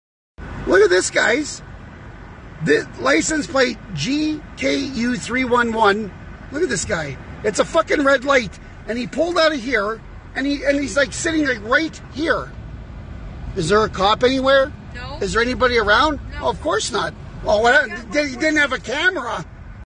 What a moron!!!!!
Why bother stopping? Entitled dink! Is there a cop in sight? Of course not! 1/21/22.